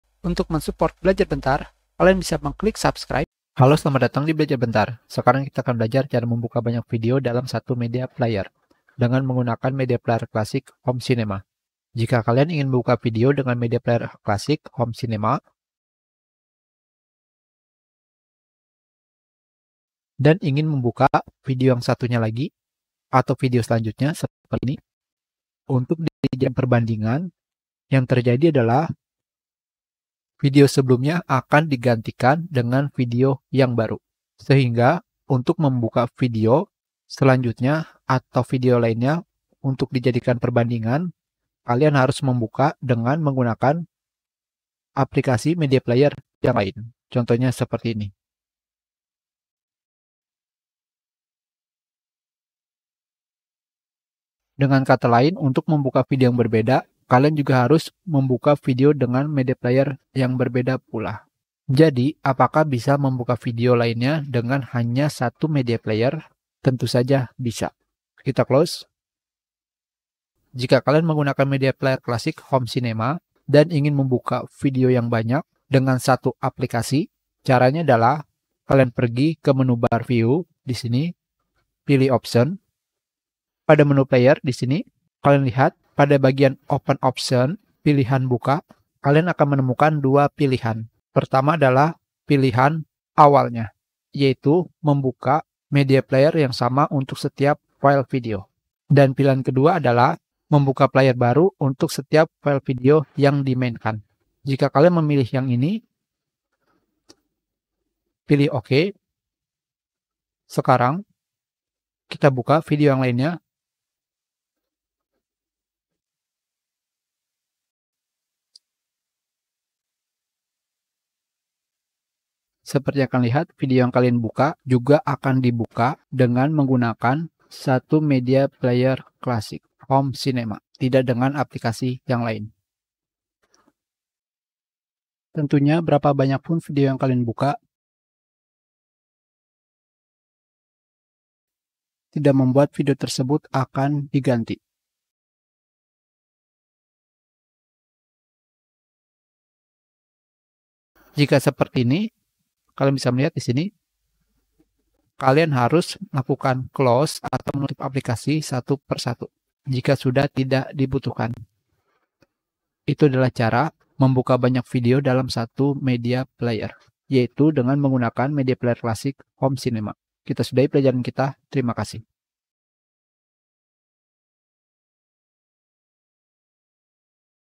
Membuka Banyak Video Dalam Satu Media Player Dengan Media Player Classic Home Cinema(MPC-HC)
Sekarang kita akan belajar cara membuka banyak video atau memainkan banyak video dalam satu media player dengan menggunakan Media Player Classic Home Cinema.

Media Player Classic Home Cinema adalah aplikasi yang banyak digunakan orang, dan selalu diupdate mengikuti perkembangan format video. Pada aplikasi pemutar media player classic ini kalian bisa memilih dari beberapa video renderer, codec, audio, dan pemutar default dari video.

Format container video yang disupport: .mkv .webm .mp4 .mov .avi .ogm .m2ts .ts .mpg .3gp .wmv .mxf .flv .rm .rmvb .dv

Format container audio yang disupport: .mp3 .flac .m4a .ogg .wv .ape .mka .opus .mpc .tak .alac .amr .ofr .tta .ra .tak .spx